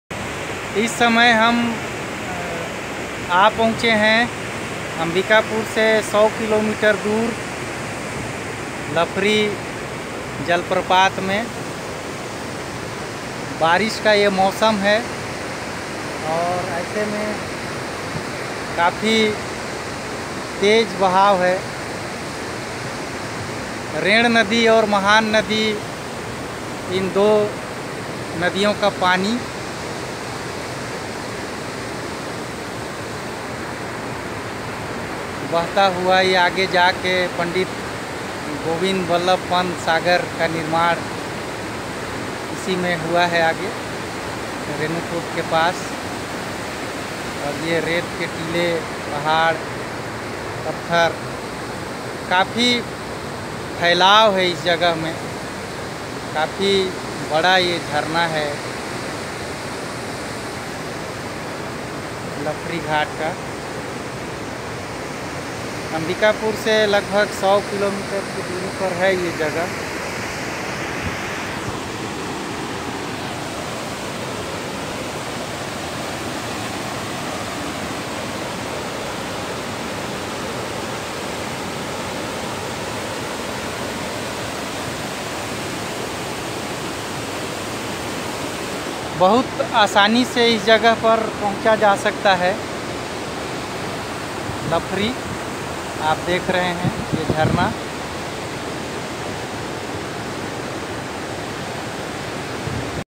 जलप्रपात लफरी,ओड़गी, सूरजपुर ज़िला,छत्तीसगढ़ # waterfall Lafari
लफरी जलप्रपात सूरजपुर जिले के ओड़गी विकास खंड अंतर्गत बेहतरीन पर्यटन स्थल है।इसकी दूरी सूरजपुर जिला मुख्यालय से लगभग पचास किलोमीटर है